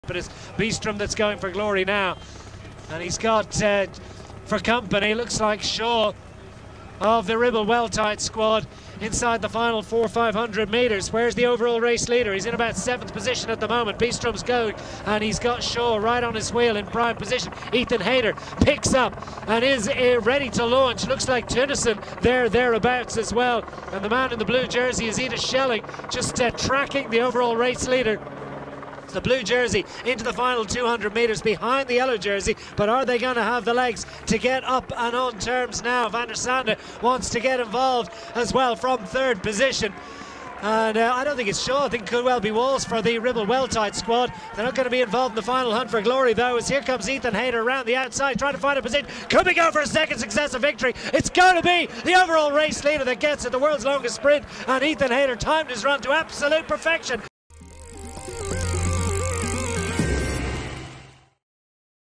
Awesome Ethan Hayter power numbers at Tour of Norway
INEOS Grenadiers rider Ethan Hayter was on fire at the 2021 Tour of Norway. Take a look at the awesome power numbers he produced to win Stage 2.